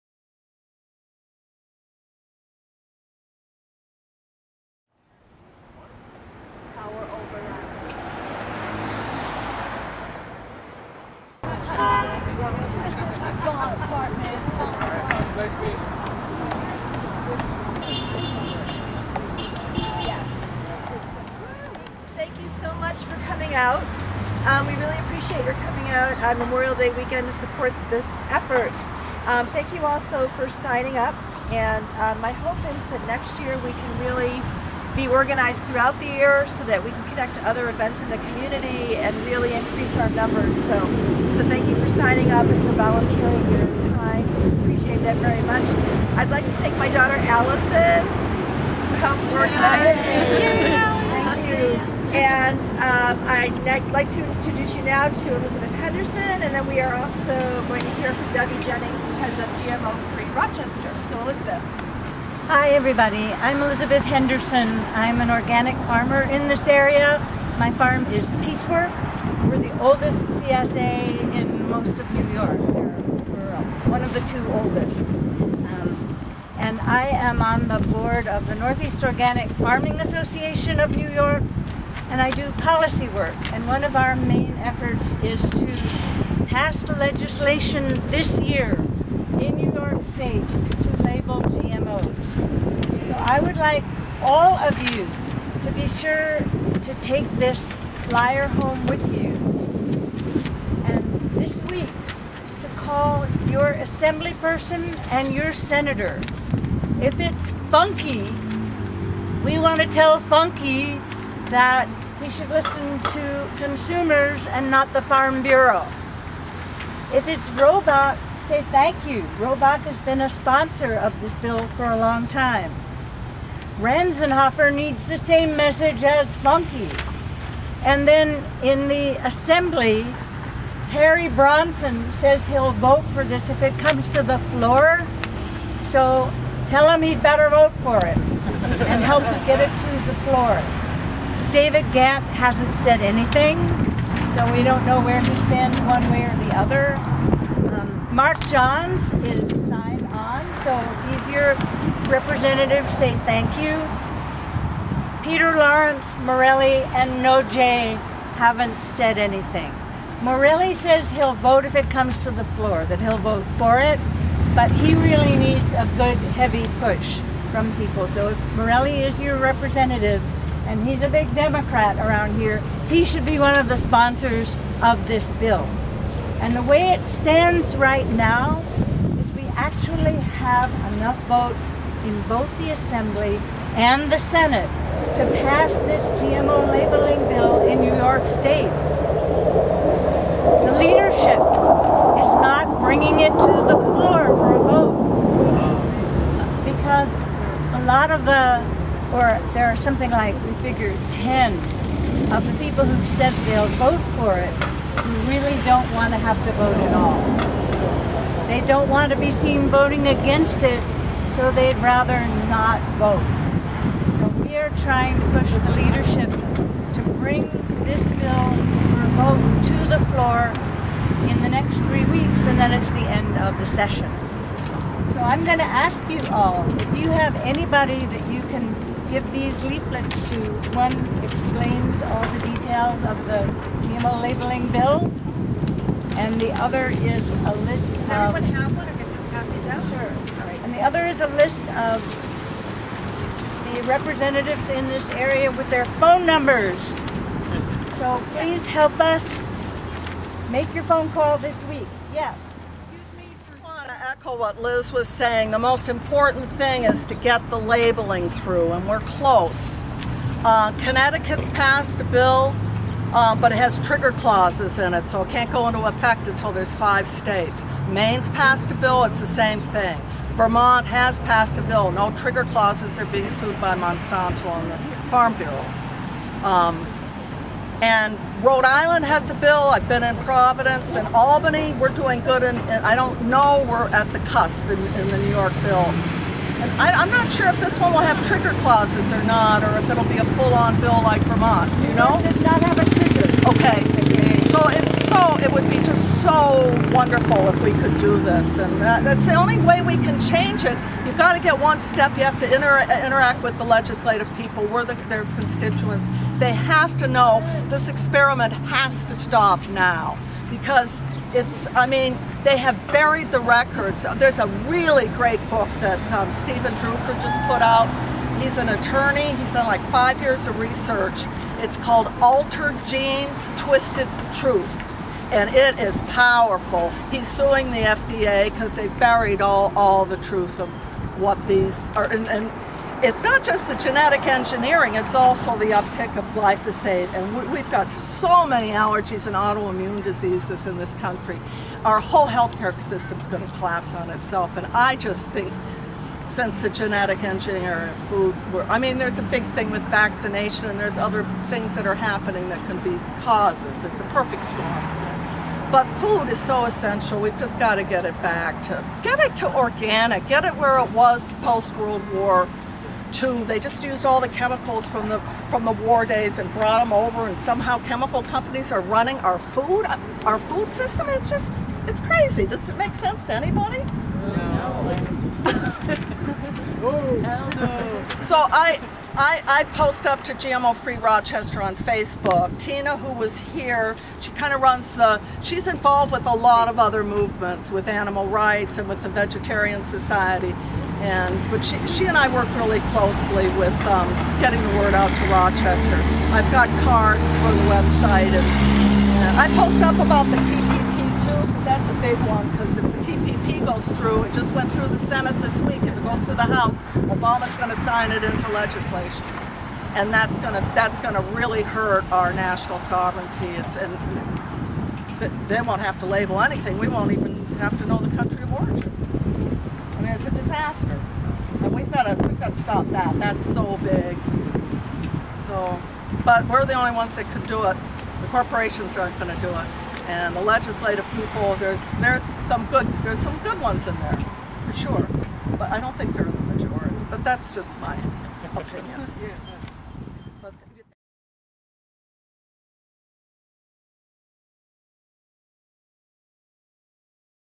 March Against Monsanto 2015
Rochester NY participates in international effort to require labels on Genetically Modified food